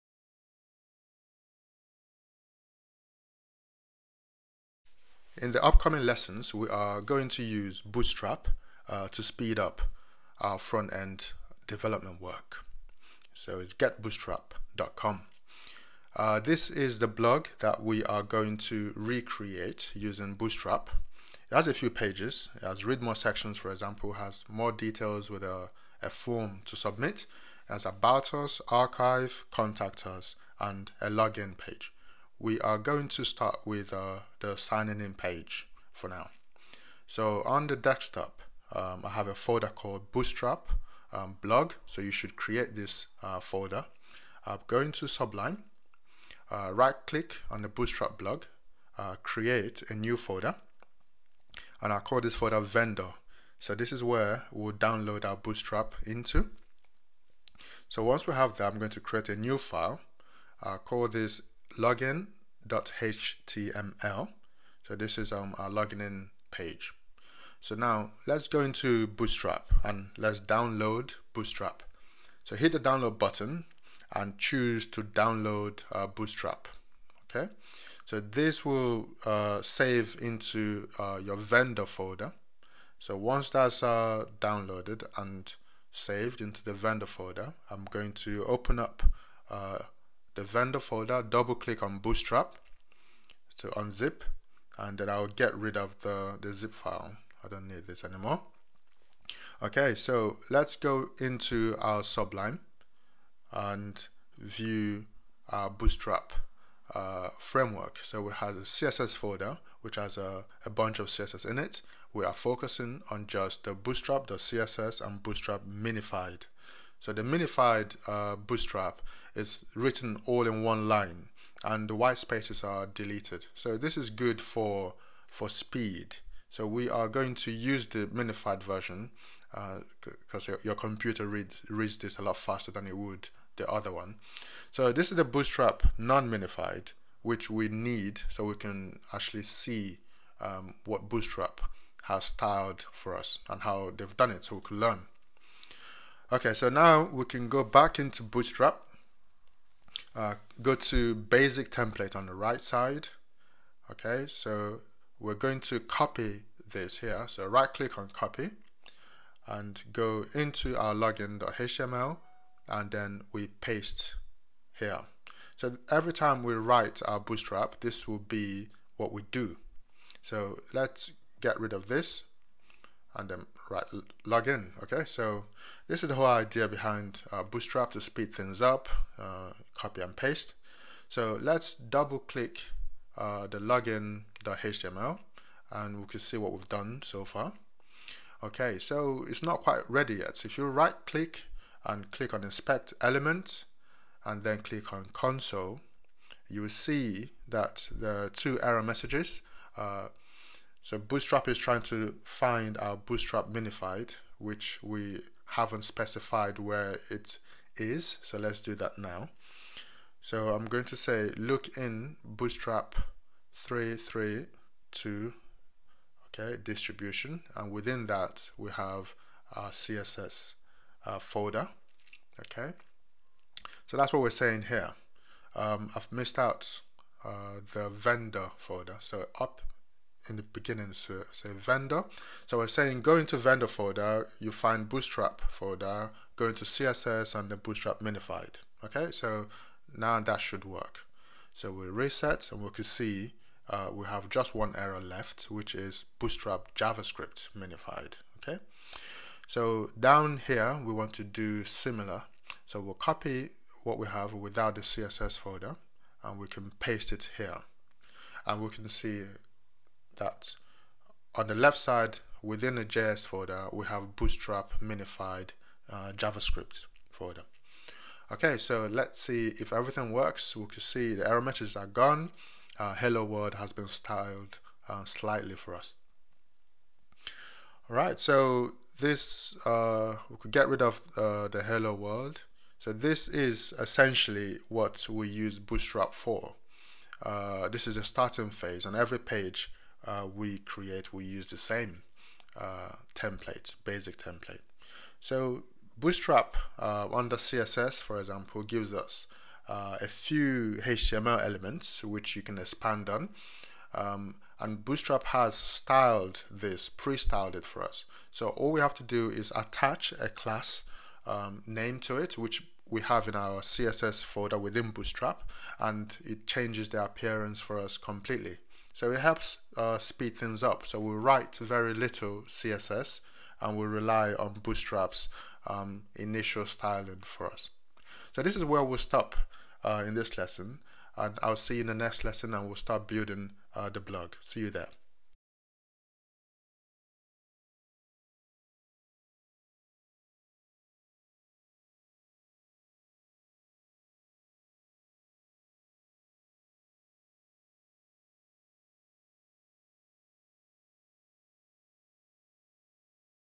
Bootstrap 3 Tutorials [COMPLETE] - Building a blog video for beginners tut # 1
These series of videos introduce bootstrap framework, we are going to create a blog right from
scratch, in each lesson we are going to touch on different aspects of bootstrap and how they can
be used to speed up front-end development.